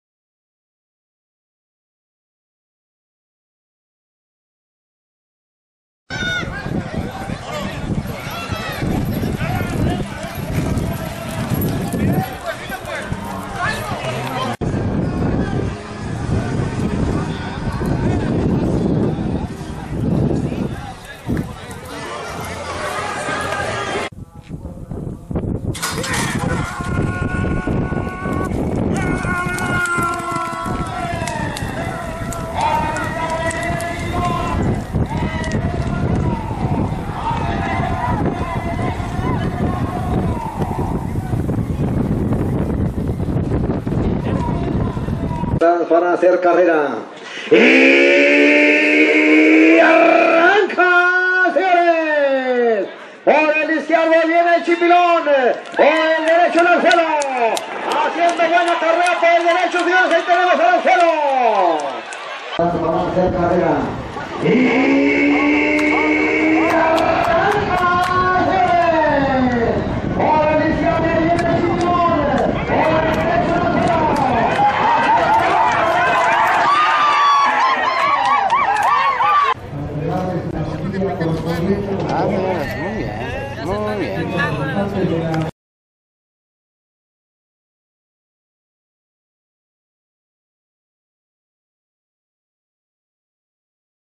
5A. CARRERA EN 225MTS EL ANZUELO VS EL CHIPILON 07 JUNIO 2014
HIPODROMO DE HERMOSILLO CARRERAS DE CABALLOS